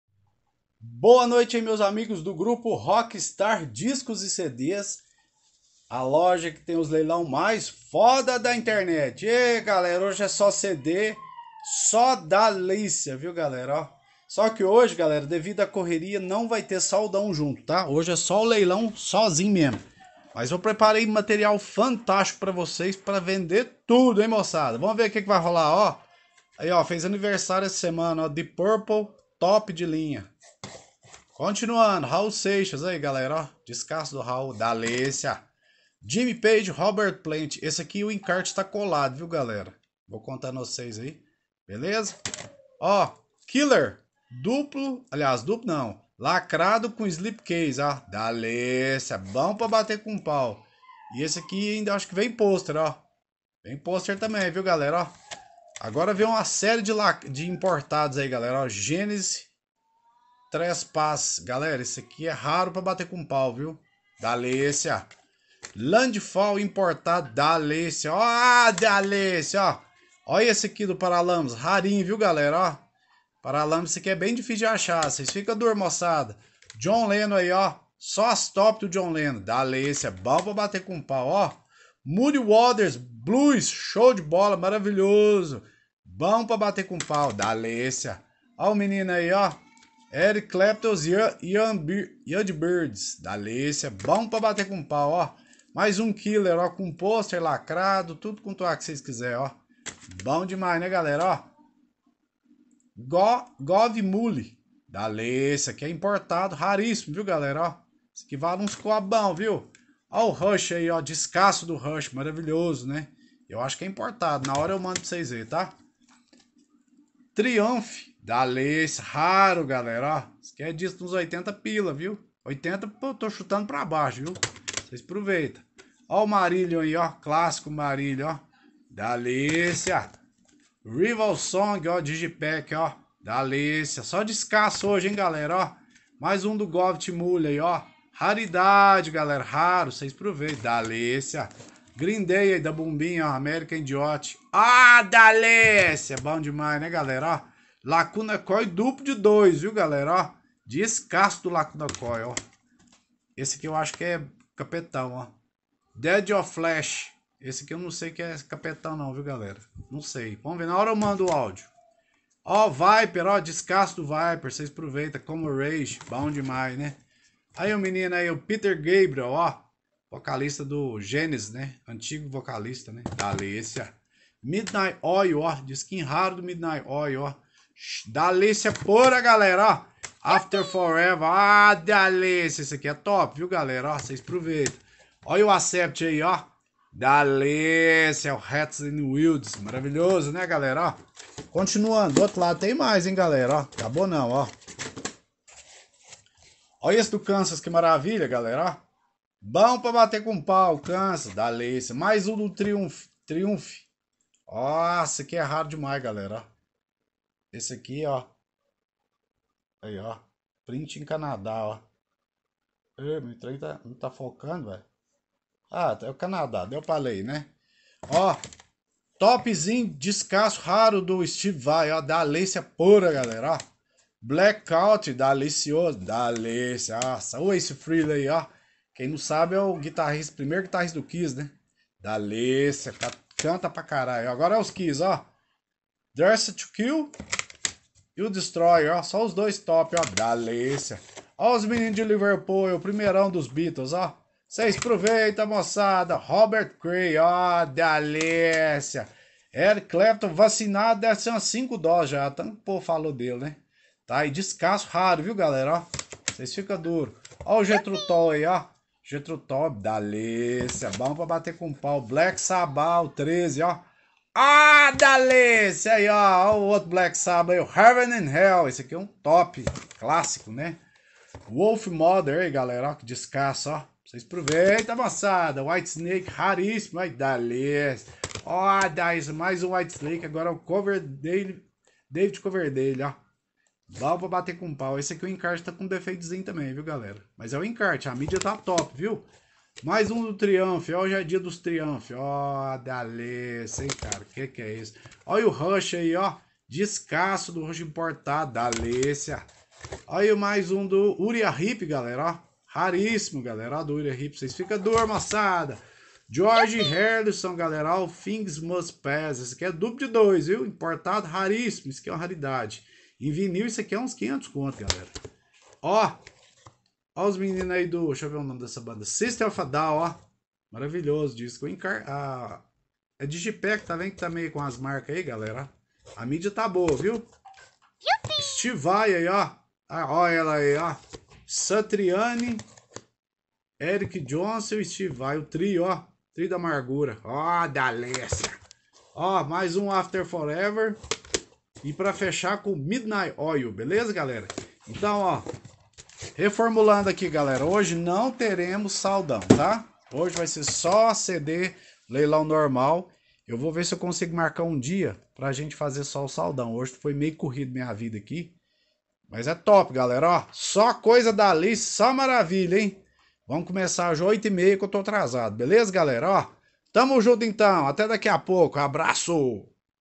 PRÉVIA DO LEILÃO DE CDS (ROCKSTARR DISCOS E CDS) - 08/06/2022
Hoje está fantástico!! Só CDs Top! Começamos as 20:30 horas! Todos convidados!!